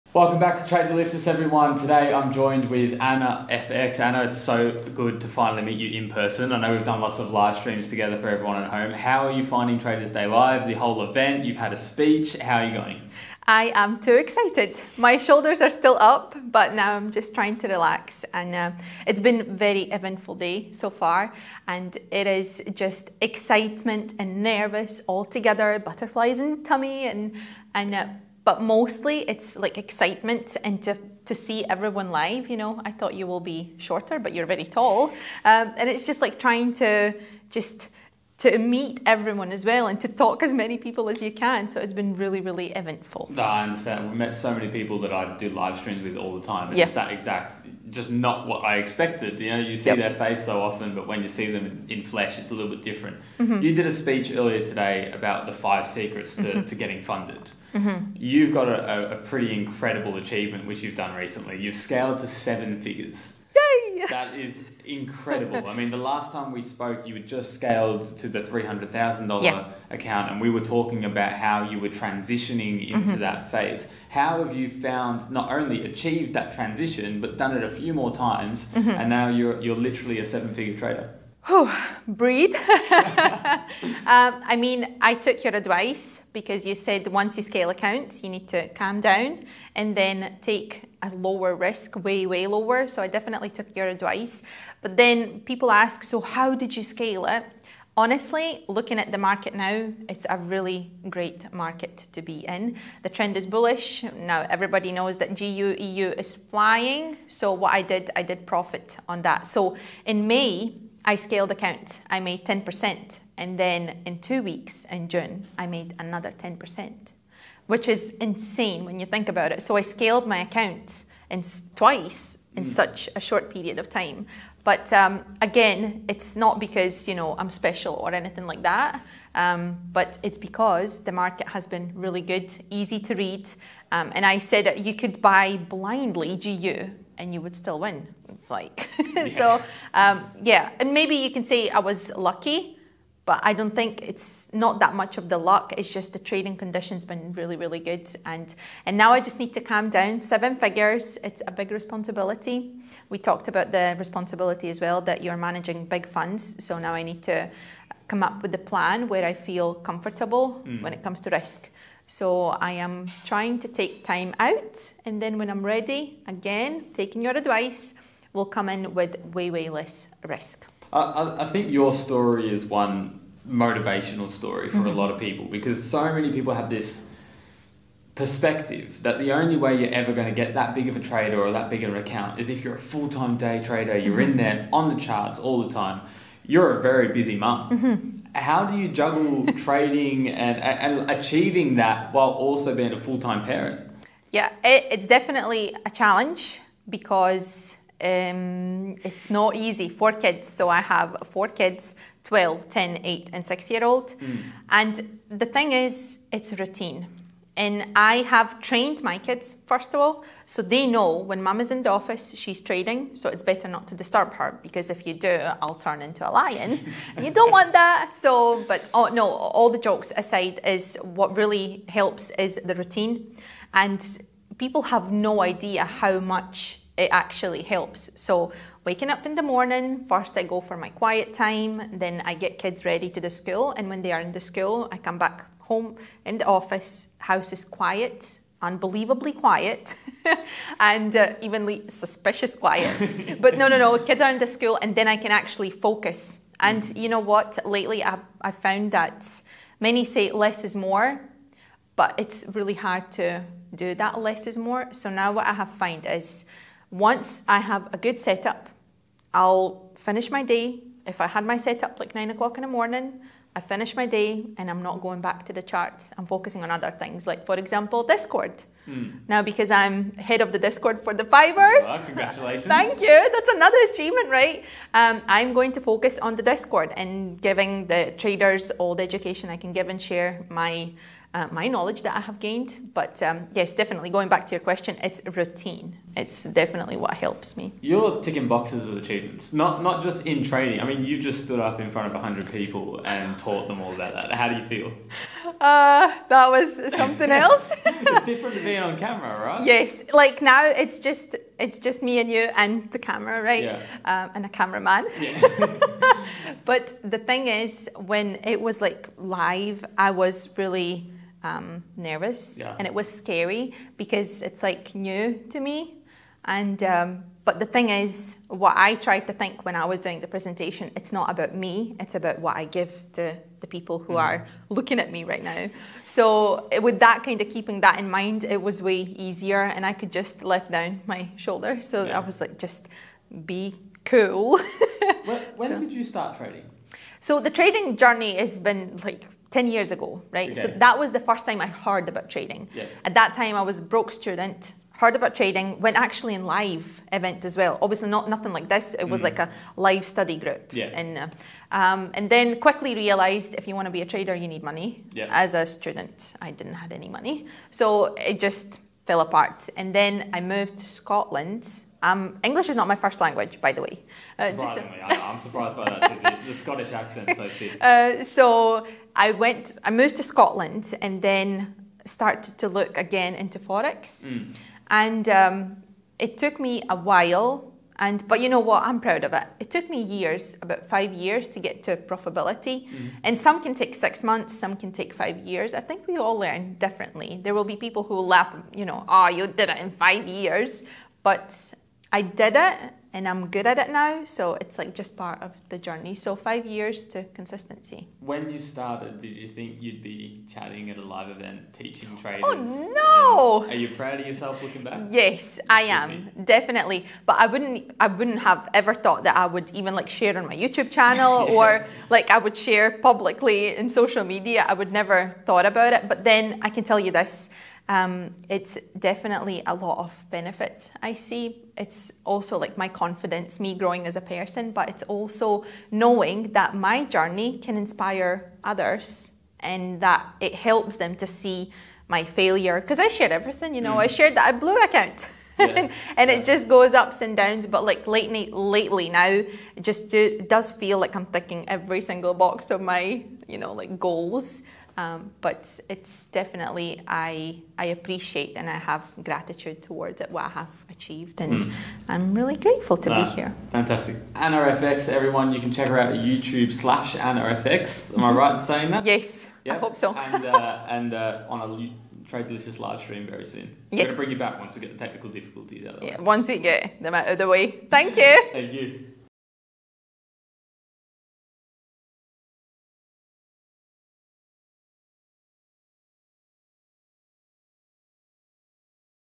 Full Time Mum Becomes Million Dollar Trader
Join us as we interview Anna Ikerte, also known as AnnaFX, a million dollar trader for the 5%ers and a full-time mom. Discover Anna's passion in the world of forex trading and valuable tips for success.

Learn how she balances her trading career with motherhood and gain insights from her experience. Don't miss this inspiring interview with a successful trader who is achieving financial independence while managing the joys of motherhood.

★ ★  CONTENTS  ★ ★
0:00 Introduction
0:14 How are you finding the event?
0:57 Scaling to 7 figures
2:50 Balancing motherhood
4:55 Personal achievements
6:38 Looking back
7:59 Final Remarks

Anna's YouTube: @anna.ikerte

👉 Forex or Indices trader looking to get funded?
👉 Stock trader wanting more buying power?